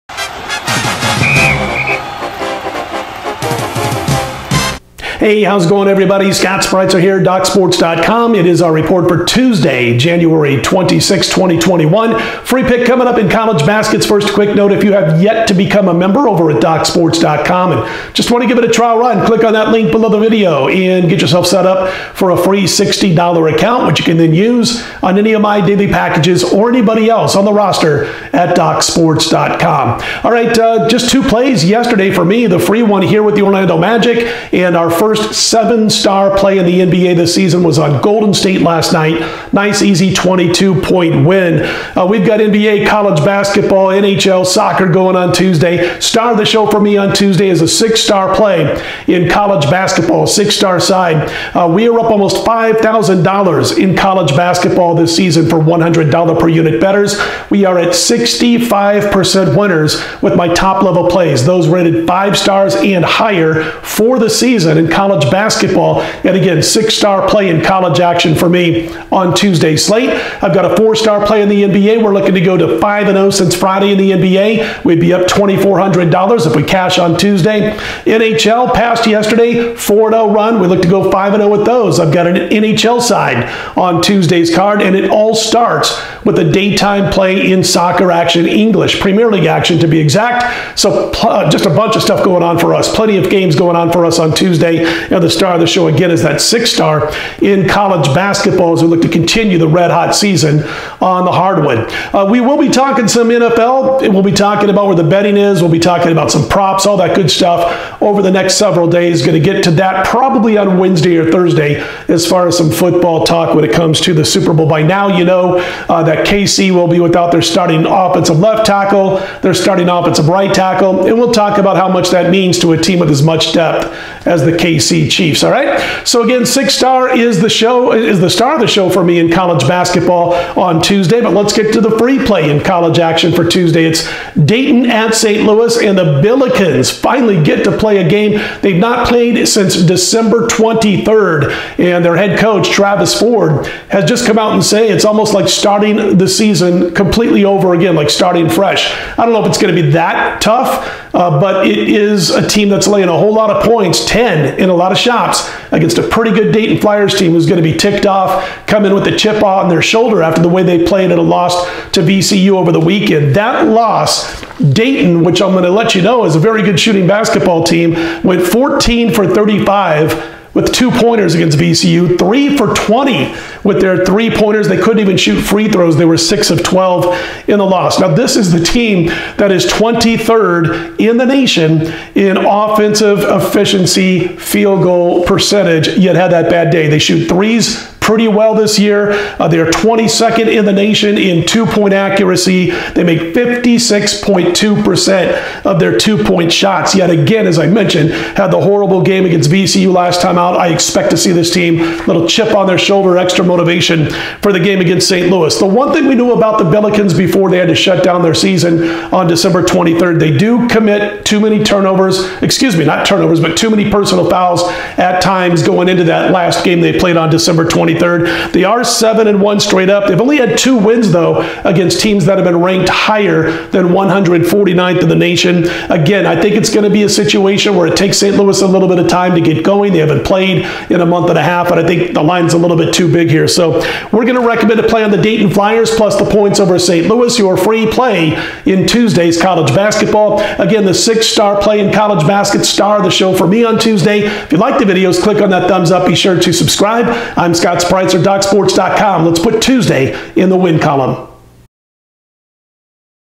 Dayton Flyers vs Saint Louis Billikens Prediction, 1/26/2021 College Basketball Pick & Betting Odds
Dayton vs Saint Louis Prediction 1/26/21. The Dayton Flyers travel to St. Louis, MO to face the Saint Louis Billikens at 8:30PM EST at the Chaifetz Arena.